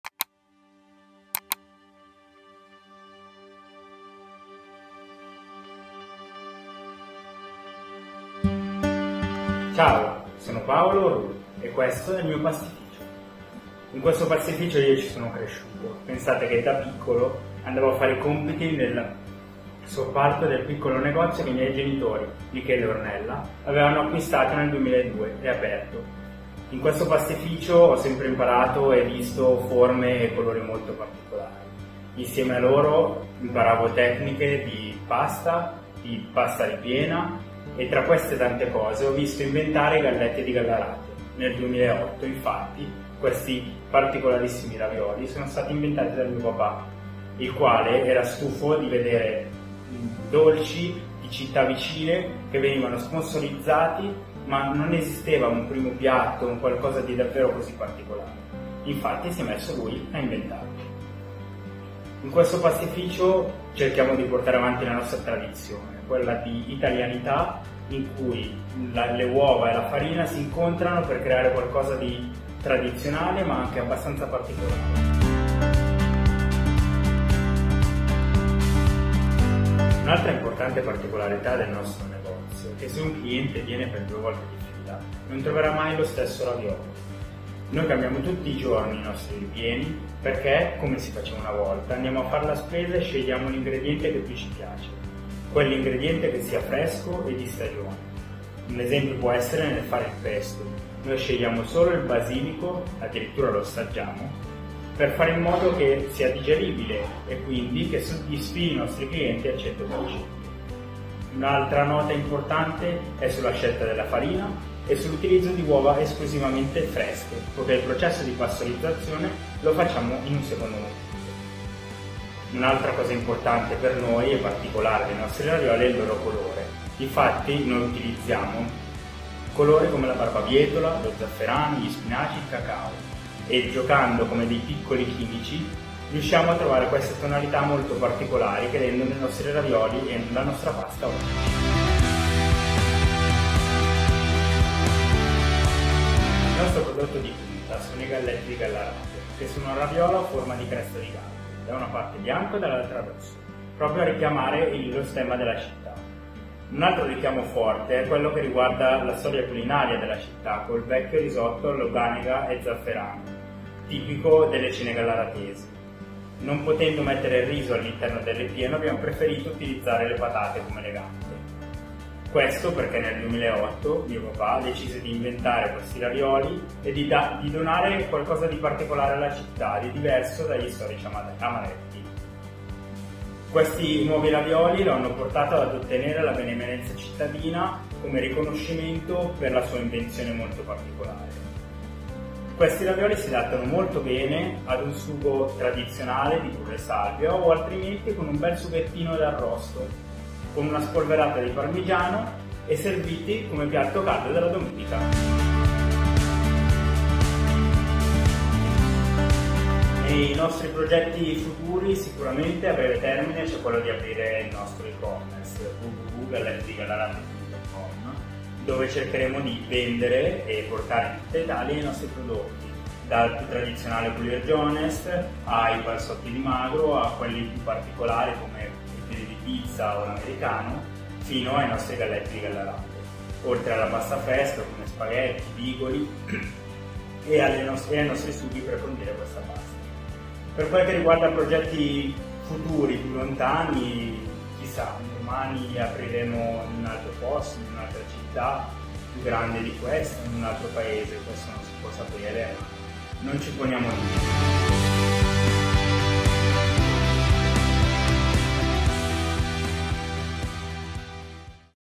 ITALIANBEES_Galletti di Gallarate
Ad ogni città il suo dolce tipico. E per chi preferisce il salato? Le Api hanno volato fino a Gallarate, la città  italiana con i due galli nello stemma, per scoprire i ravioli bianchi e rossi dell’omonimo pastificio: i Galletti di Gallarate. Qui, Paolo Orrù e la sua famiglia celebrano la tradizione italiana della pasta con semplicità,  attenzione e fantasia, offrendo un tour di sapori che spazia dalla luganega e lo zafferano fino a ripieni  innovativi come patatine, wurstel e ketchup.